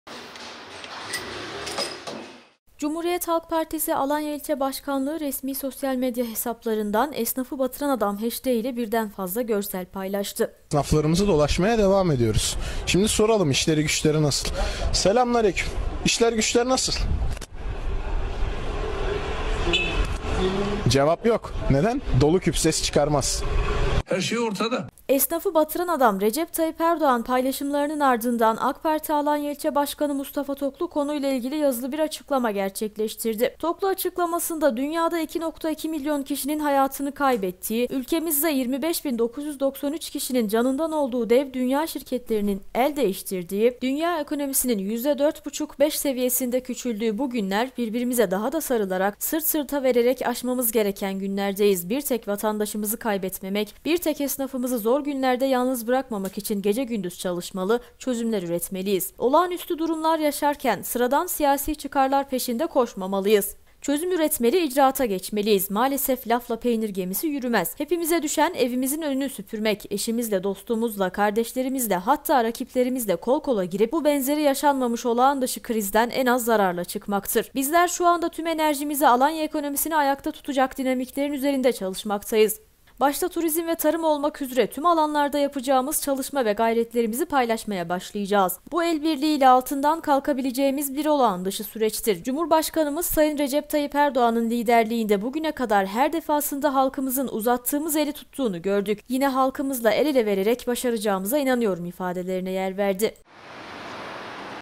TOKLU’DAN CHP’YE YANIT
Toklu yazılı açıklamasında “Lafla peynir gemisi yürümez” ifadelerini kullandı.